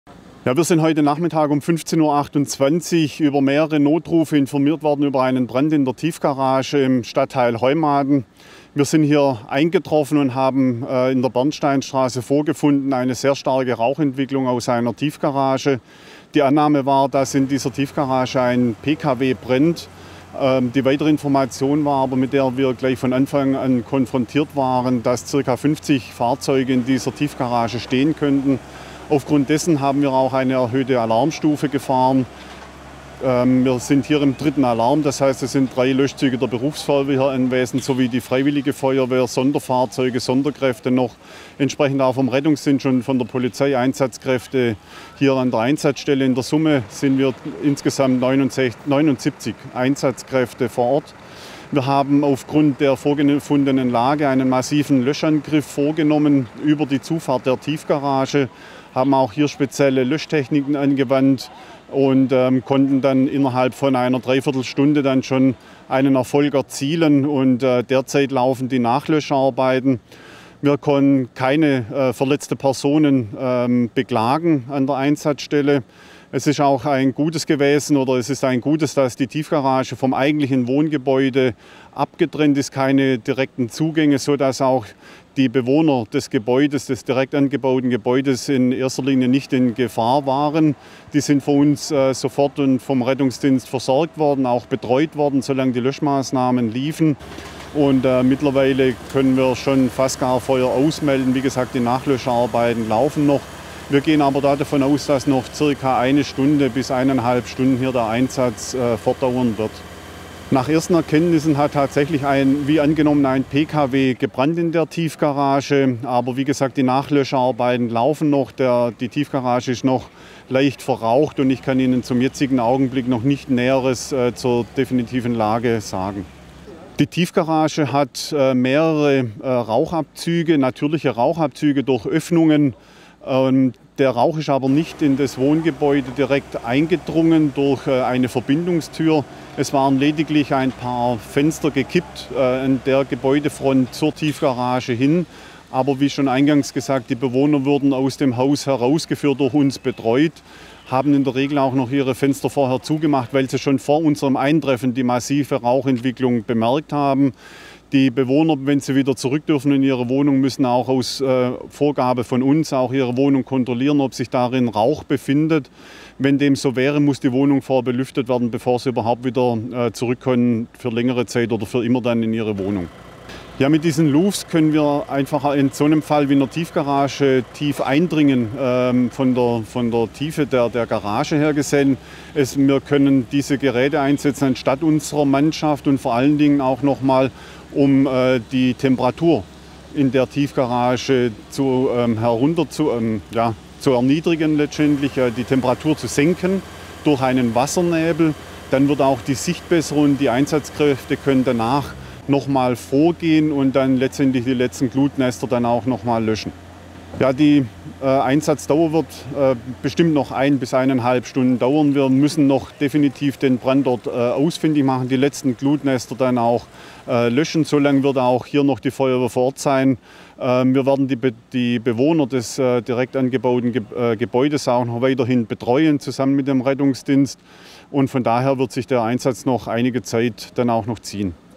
11.10.2020 - Brand in Tiefgarage
Stuttgart-Heumaden (pol) - Aus bislang nicht geklärter Ursache ist am Sonntagnachmittag  (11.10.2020) in einer Tiefgarage an der Bernsteinstraße ein Brand  ausgebrochen. Ein Zeuge bemerkte gegen 15.15 Uhr starken Rauch, der aus  der Tiefgarage quoll und alarmierte die Feuerwehr. Nach ersten  Erkenntnissen sind mehrere Fahrzeuge durch das Feuer beschädigt worden.  Die Ermittlungen zur Brandursache dauern an. Der Schaden beträgt nach  bisherigen Schätzungen vermutlich mehrere hunderttausend Euro. Für die  Dauer der Löscharbeiten musste die Kemnater Straße gesperrt werden.